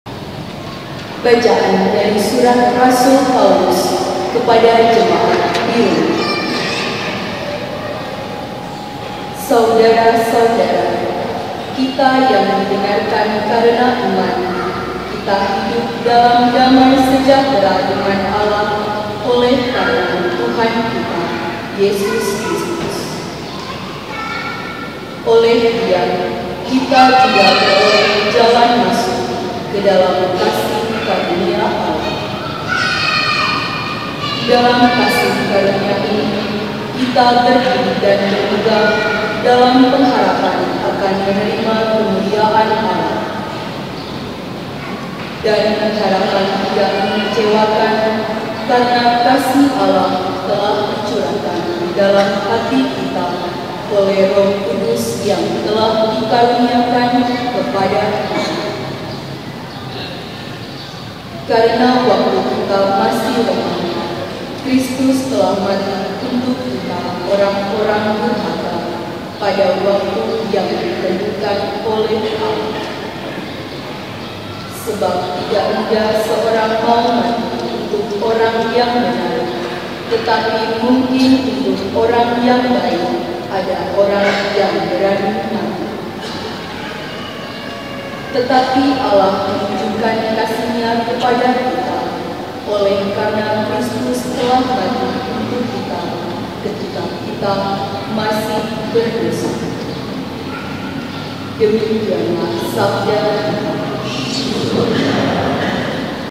Pembacaan Kedua
Pembacaan Kedua pada Misa Perdana Romo Paulus Piter di gereja Paroki Keluarga Kudus Cibinong pada Hari Minggu Tanggal 19 Maret 2017.